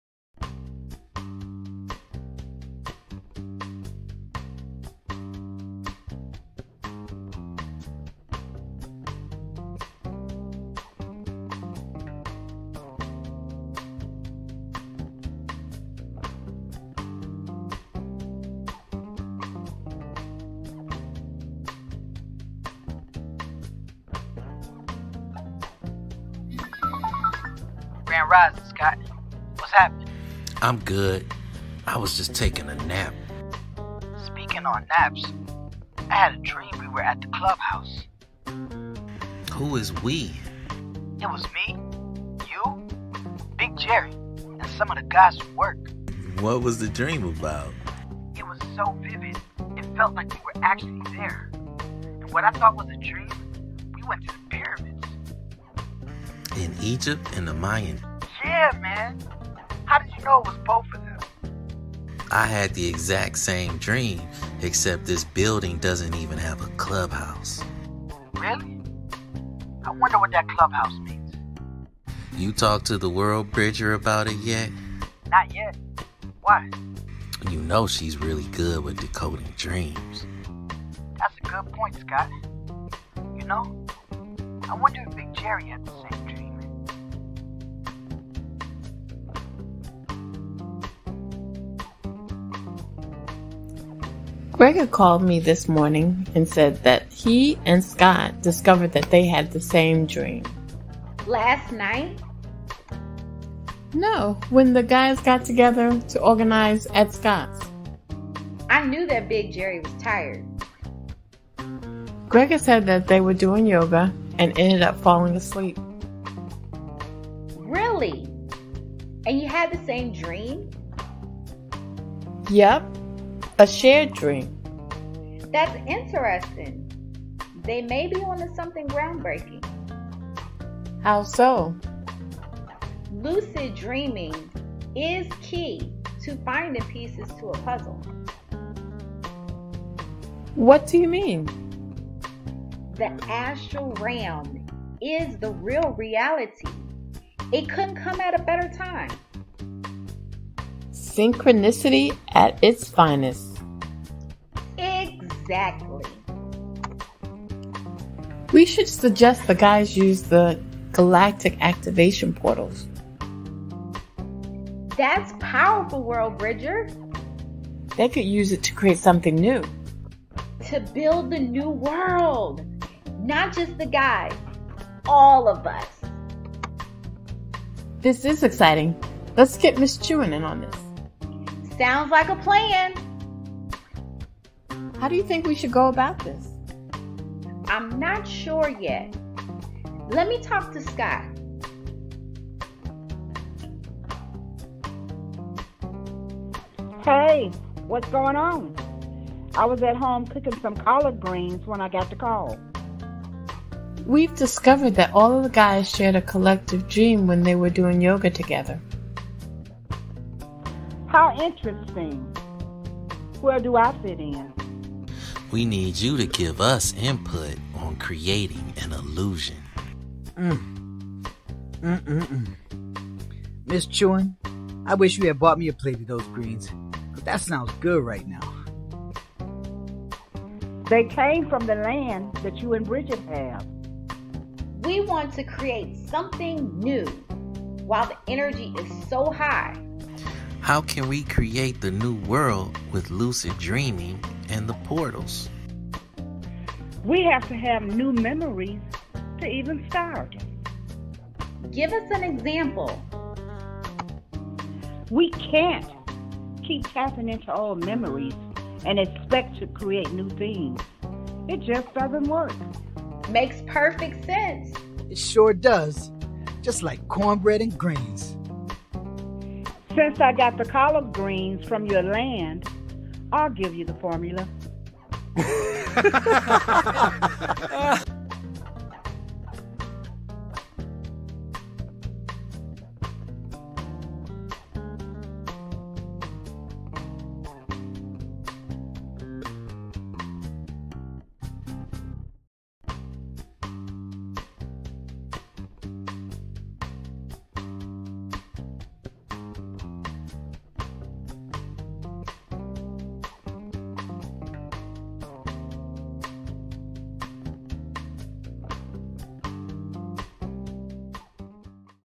New Memories
The team explores the guys dreams to see if they can capitalize on the Galactic Activation Portals (GAP) coming up soon.